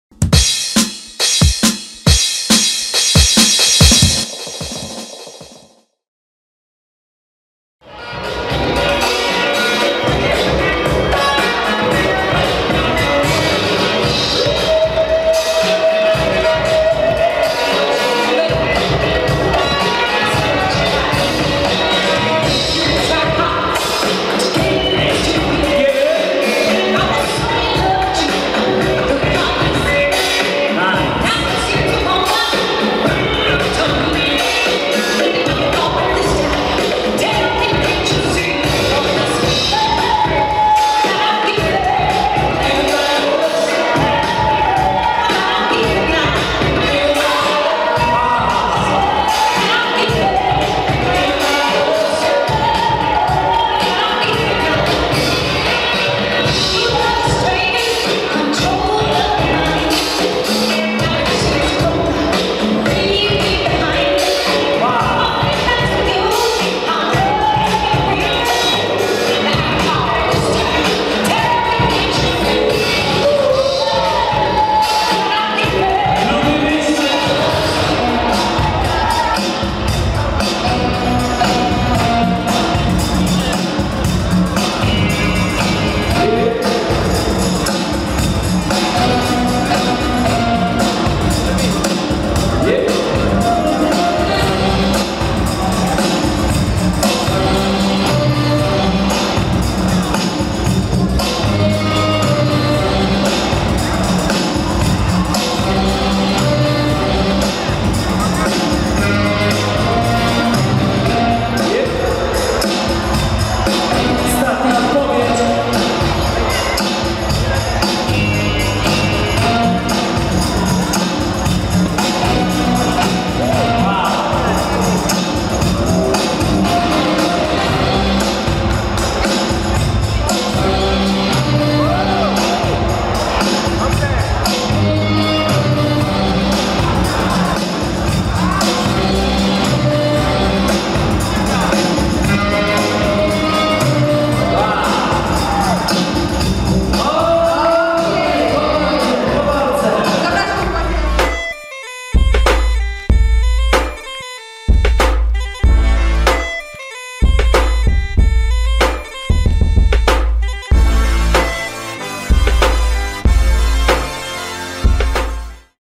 Ostrowski Styl 2018 / Finał Bboying 1vs1 do 11 lat / Bboy No Gravity vs Bboy Nicku
Ostrowski Styl 2018 / Finał Bboying 1vs1 do 11 lat / Bboy No Gravity vs Bboy Nicku / MOSiR Ostrów Mazowiecka / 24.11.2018.